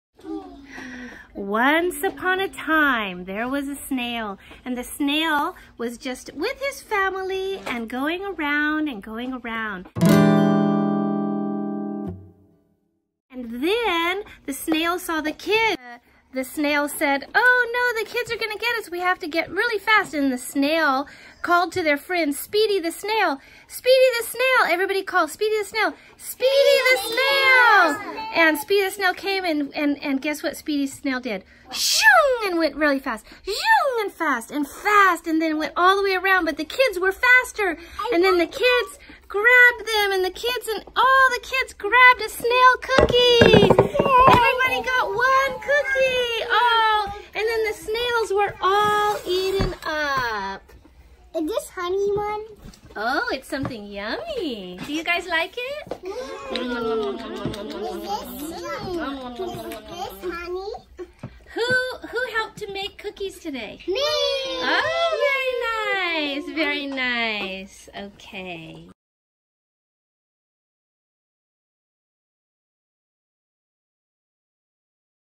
Treat’s Cafe Tuesday at our KiDo Kids Yoga Preschool.
Snail 🐌 Cookies 🍪 and Story time anyone?  Every Tuesday is My World (social studies) with a variety of themes.
or at Instagram and Facebook @KiDoKidsYoga